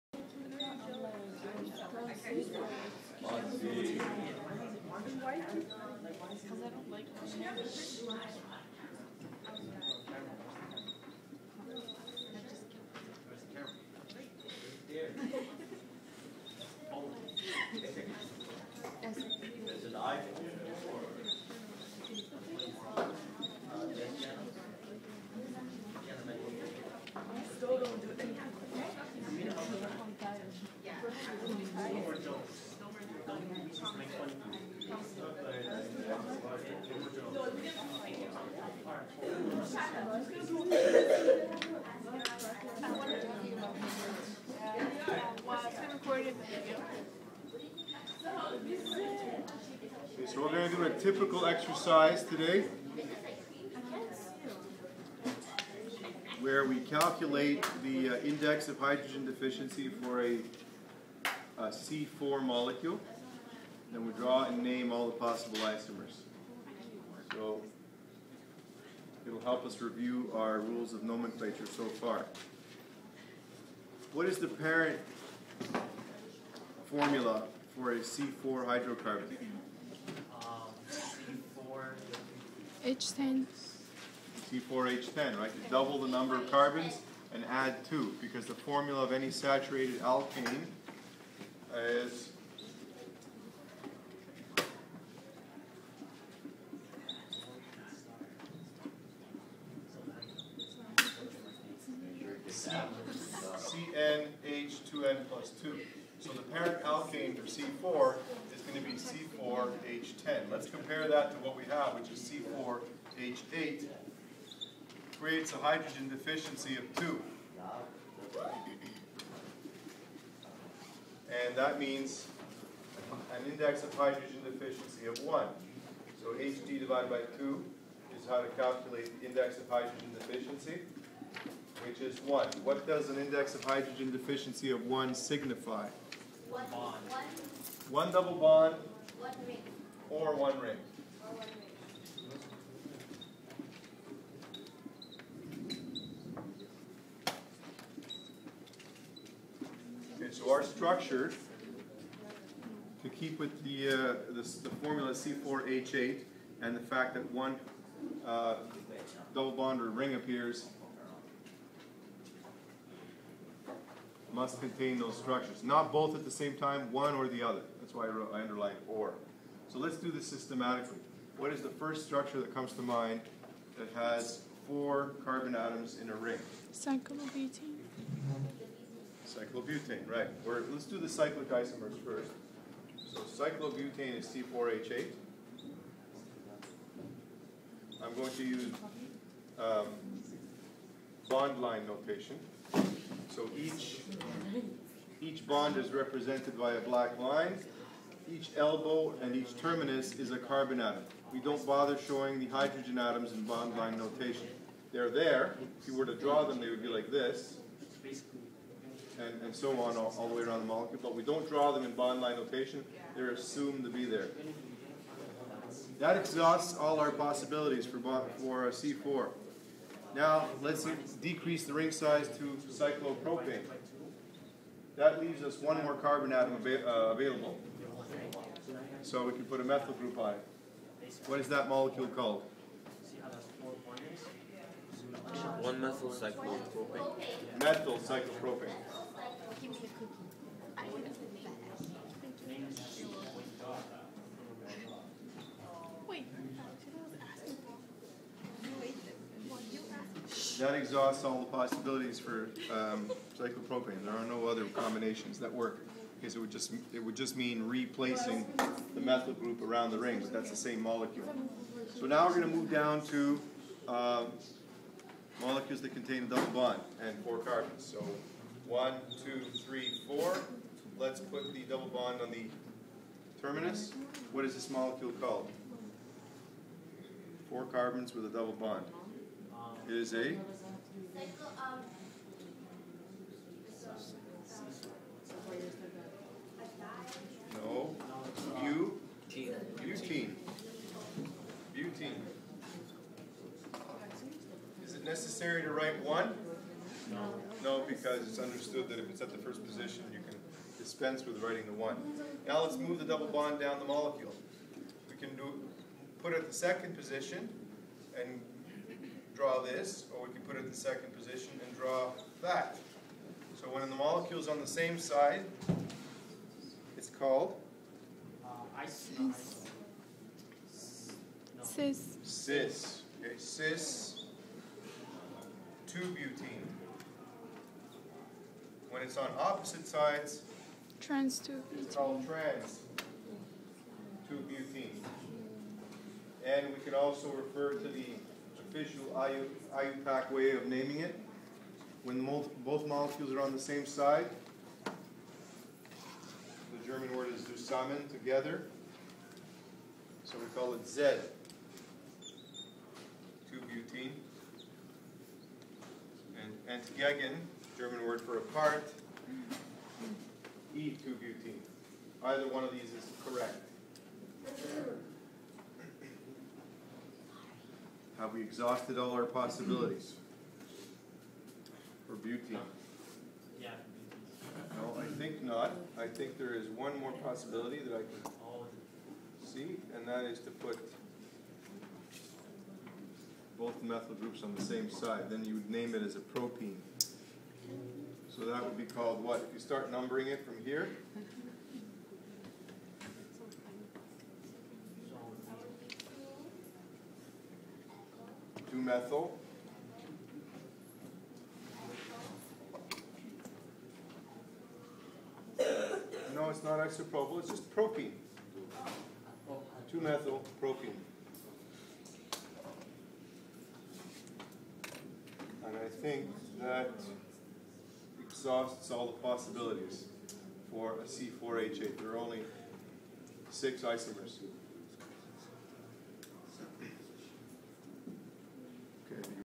Nomenclature: Drawing and Naming the isomers of C4H8 in bond-line notation.
The index of hydrogen deficiency determines the amount of unsaturation in an organic molecule and helps to narrow down the number of possible isomers. In this example, an index of hydrogen deficiency of 1 indicates the presence of one ring or one double bond. Mr. Bozzi proceeds to draw and name the isomers. This video was recorded September 21, 2011 at Father Henry Carr High School. There is another video in this collection where a larger number of isomers is listed with the formula C6H12.